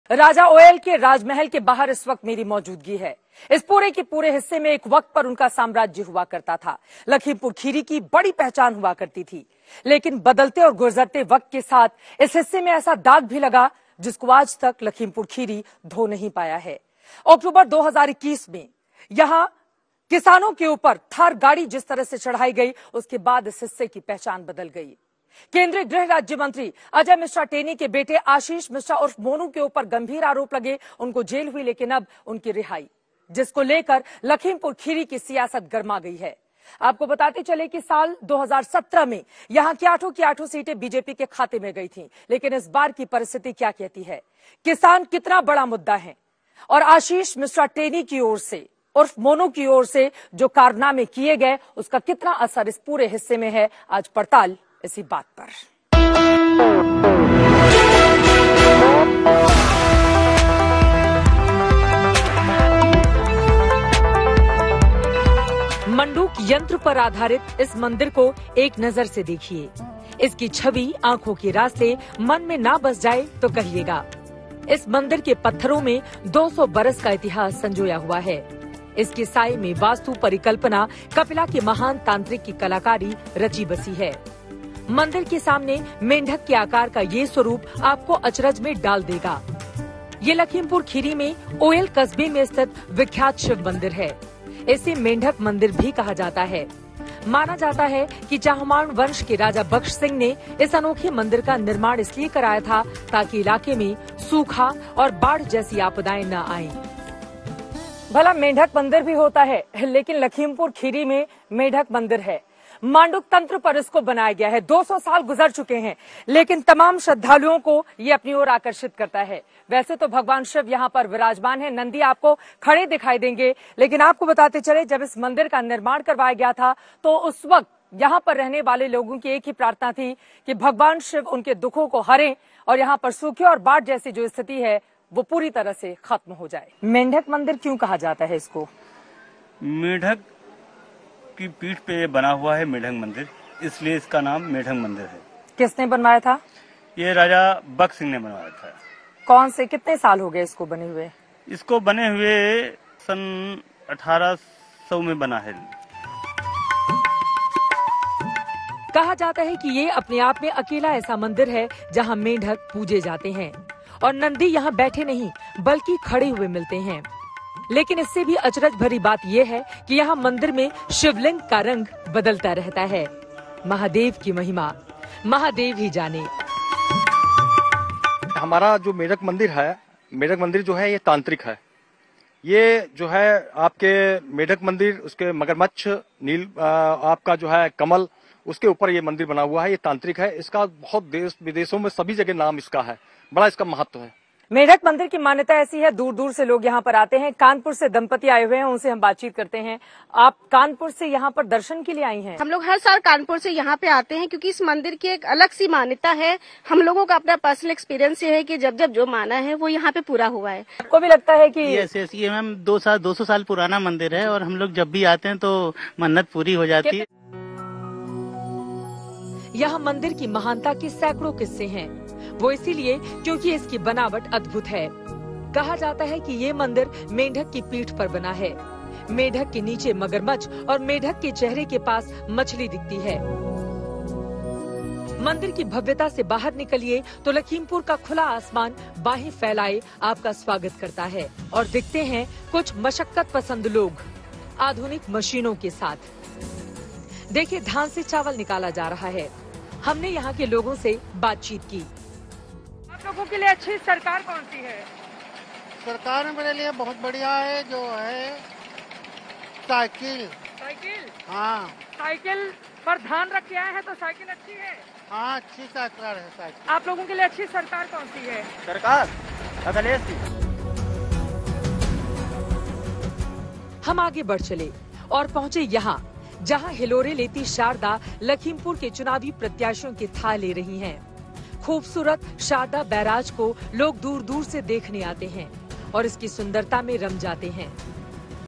Lakhimpur-Kheri: लखीमपुर खीरी के मेढक मंदिर की कहानी, 200 साल पुराना है मंदिर का इतिहास।
लखीमपुर खीरी (LakhimpurKheri) में करीब 200 साल पुराना मेढक मंदिर अपनी महिमा के लिए प्रसिद्ध है. दूर-दराज़ से लोग इस मंदिर में दर्शन के लिए आते हैं. लेकिन पिछले दिनों लखीमपुर किसान आंदोलन और आशीष मिश्रा को लेकर भी सुर्खियों में आया. देखिए चुनावी नब्ज़ टटोलती E-Bike Reporter की ये रिपोर्ट